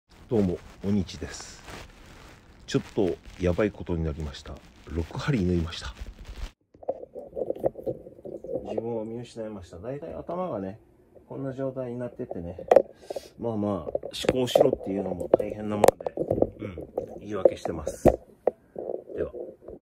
頭をホッチキスで留められました♪
朝方3時半から4時位の間に吐き気が込み上げてきました。便意もありました。
　立ち上がれない程で、四つん這いで玄関まで向かって立ち上がったと思ったら、意識が無く頭から血を流して倒れてました。
　吐き気が再び来たので起きたのだと思います。
　水を吐いて、排便もしてから、流すことは忘れない。
　その場で猫達が心配する中　突っ伏しておりました。

　出血に気がついたのは、眠りから覚めて頭いてぇからで、触ると血がついてる。皮膚が捲れてるのも分かる。
　チョッチやばい！　部屋に戻って救急車を呼ぶ事に。この時点で10時でした。
　病院で3時間程、様々な検査やら点滴やら、手術をされながら、タクシーで戻ってきました。

　入院も勧められましたが、断る！

　やらなきゃいけないことは沢山あります。